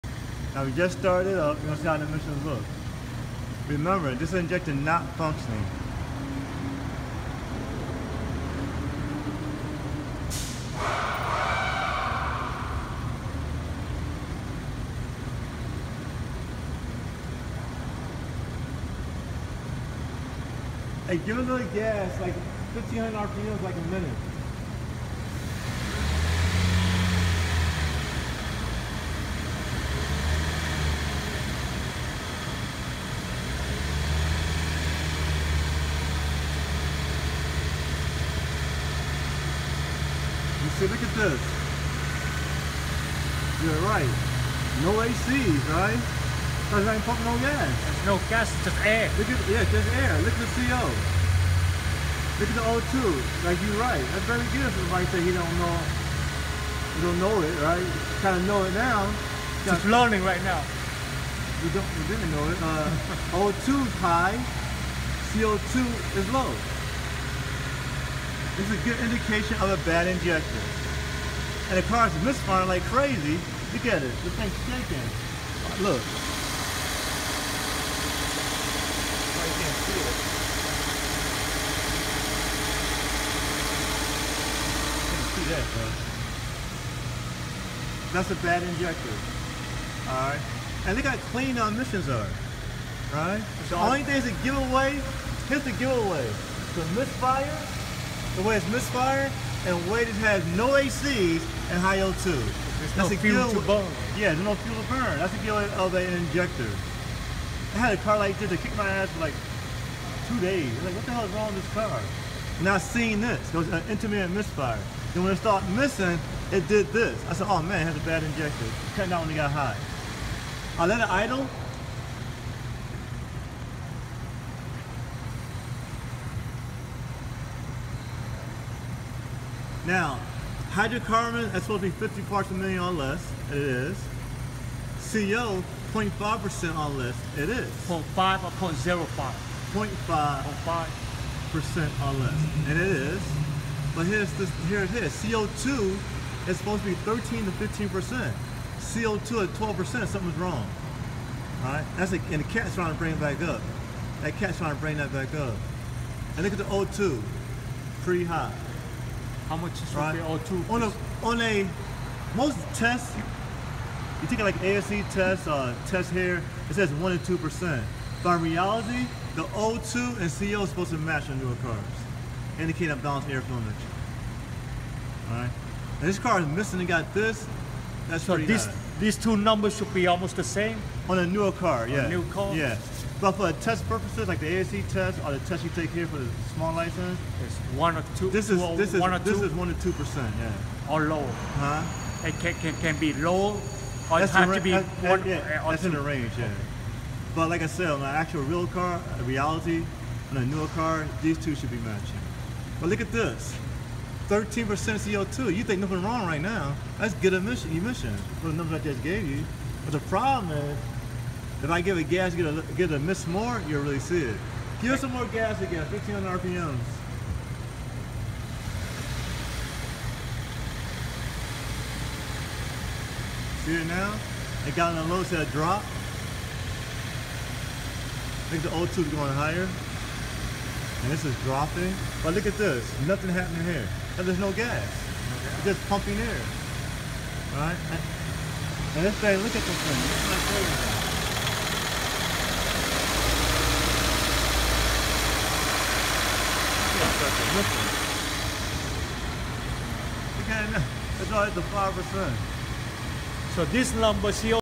Misfire Bad Injector Take a look at the Emission Readings
This vehicle had a non functional Injector causing s misfire. Look at the emission readings and see how they are different from ignition misfire